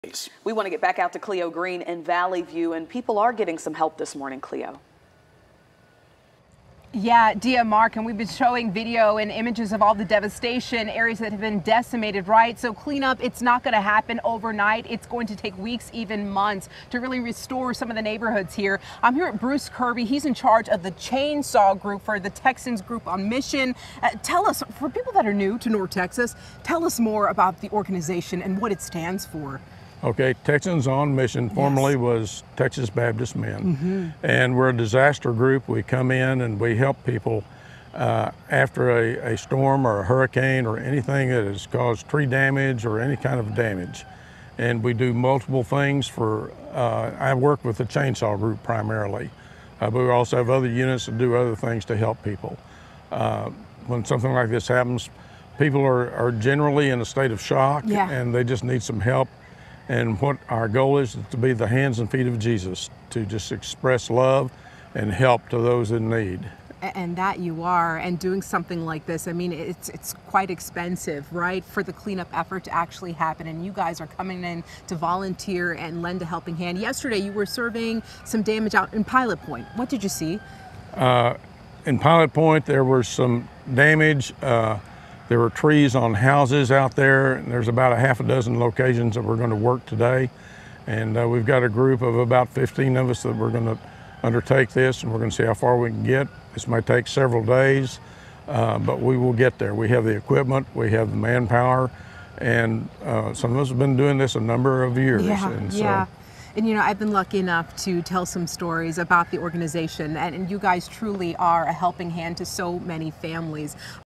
Nonprofits help Valley View, Texas recover from deadly tornado
Nonprofits are on site clearing debris to help families recover from the storm.